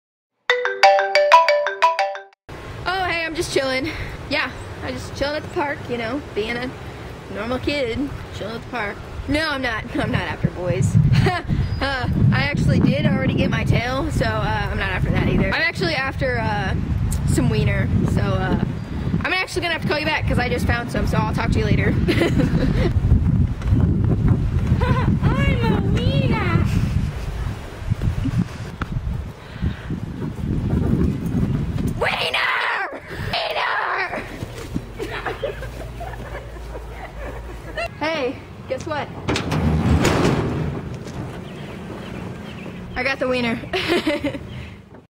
fri gets wiener!!!!
Fri spends a day at the park looking for some wiener!!